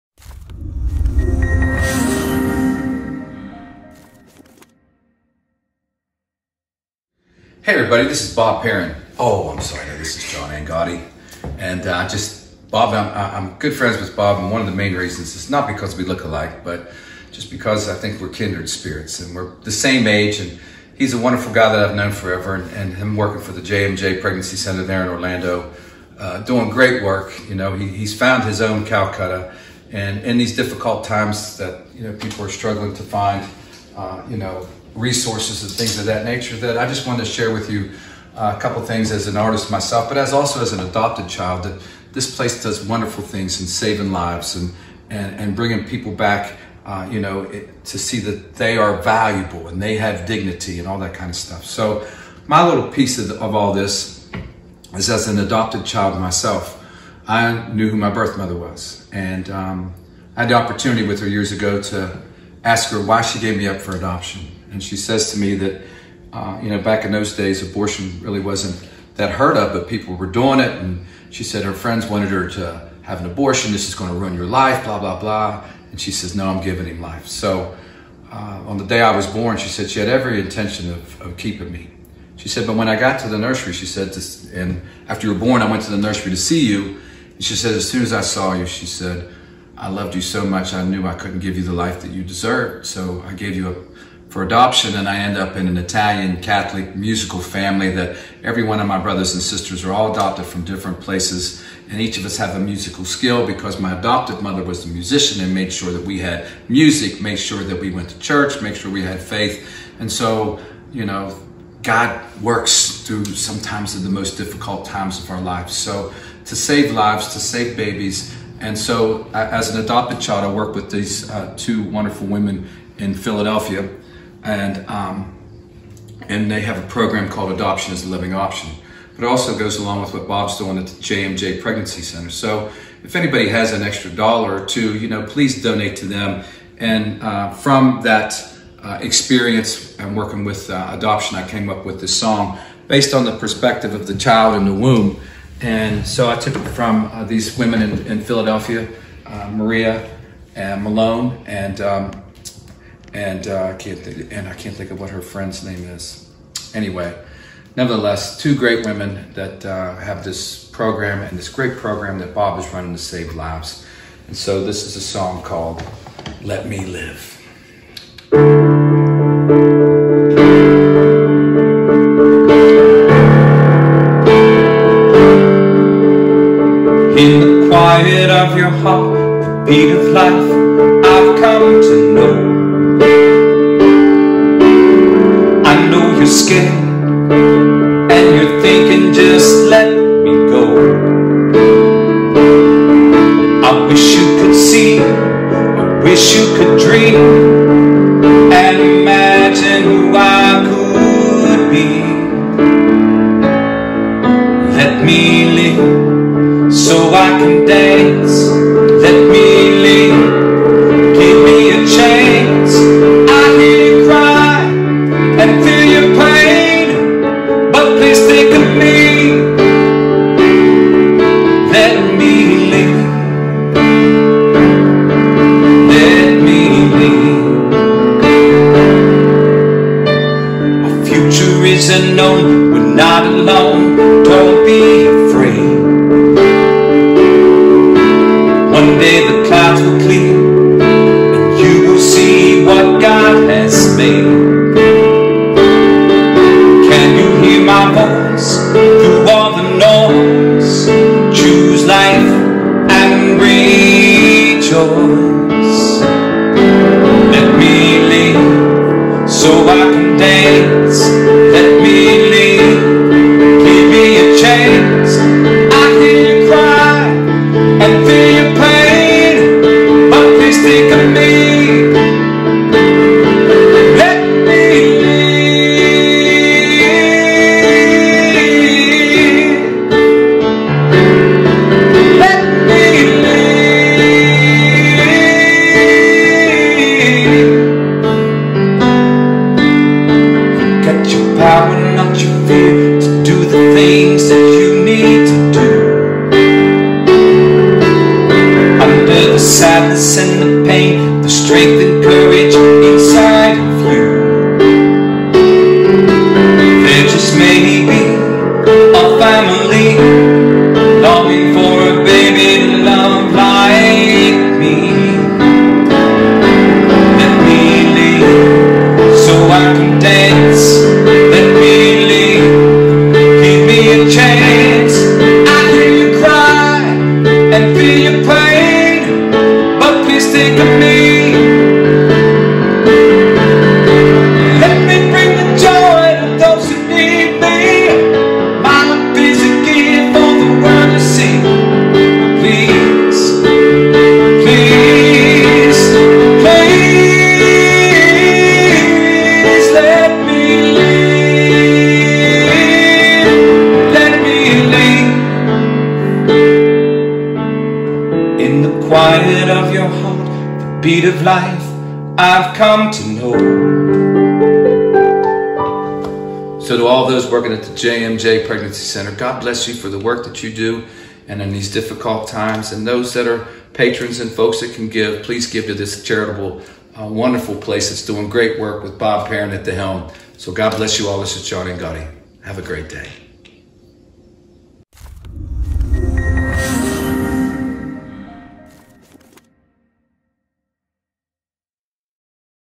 John Angotti and JMJ Pregnancy Center
John shares a  powerful story of his adoption and his encouragement for staff, volunteers, and  donors of JMJ Pregnancy Center. He ends with an amazing song that you will not want to miss.